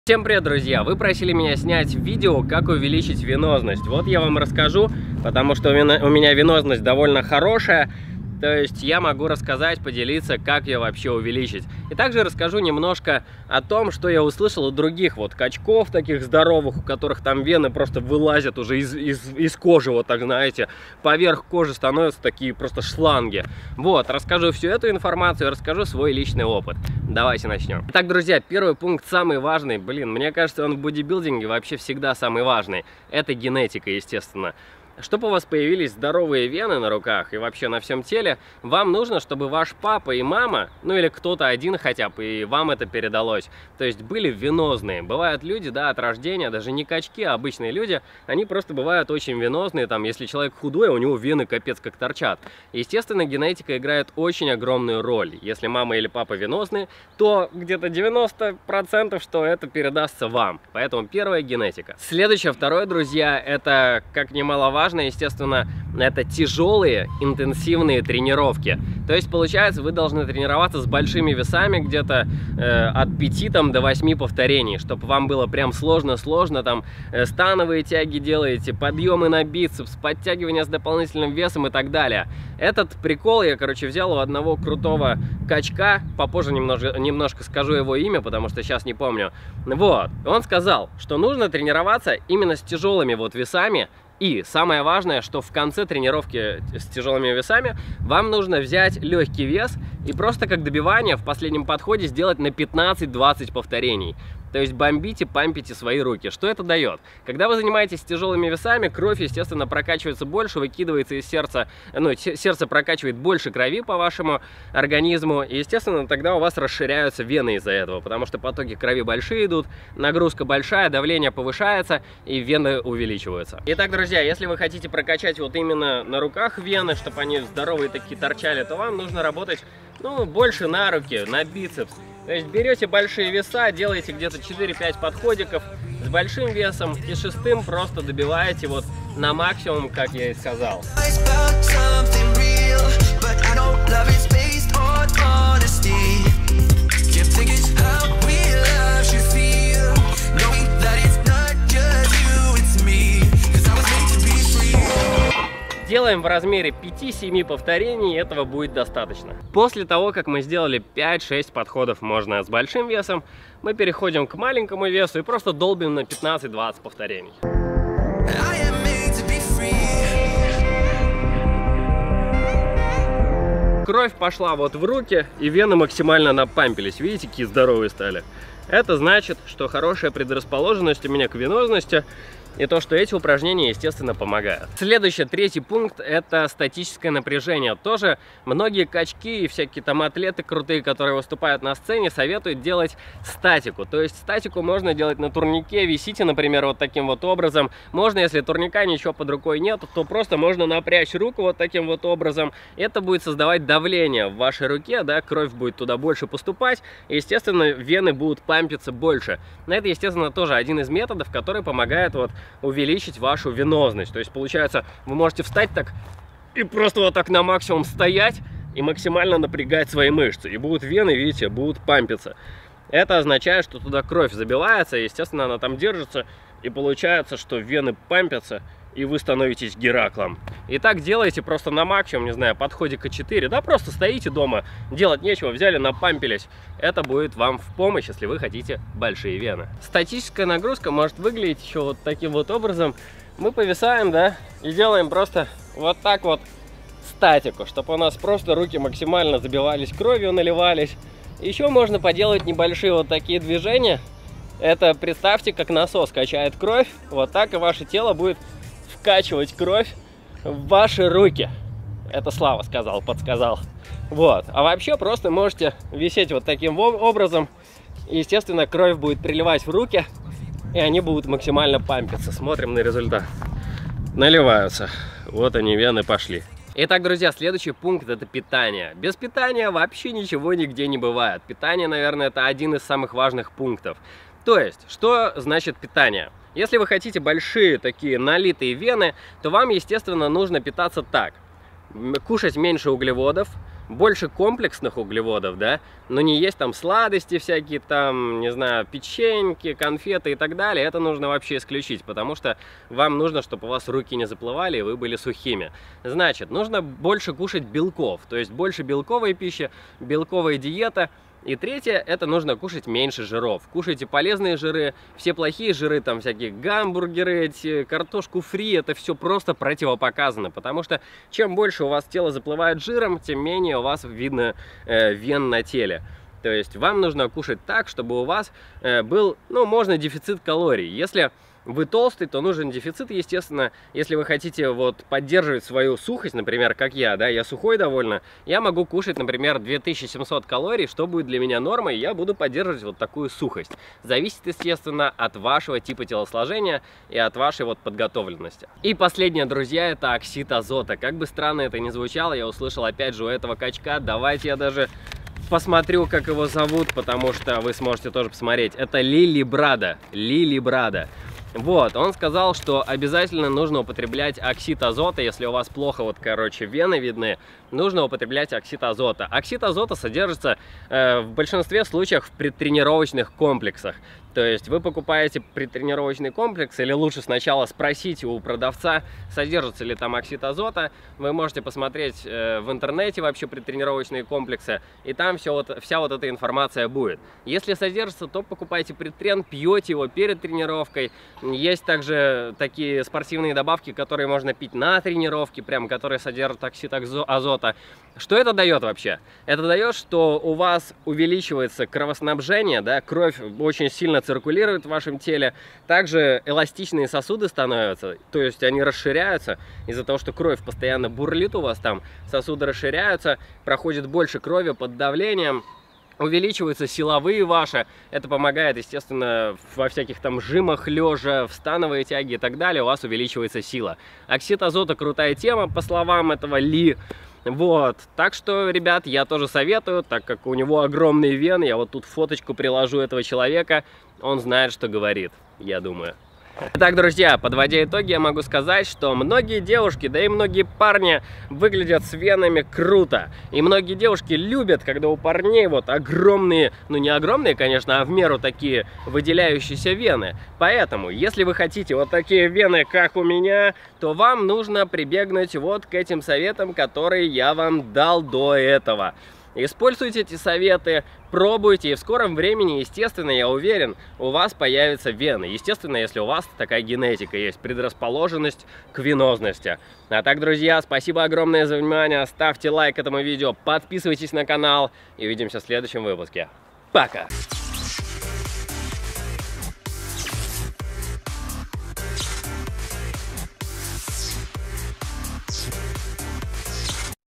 Как Накачать Большие Вены?!
Как стать венозным? Многие люди этого хотят. Не знаю по каким причинам, но хотят. Поэтому ловите такое видео!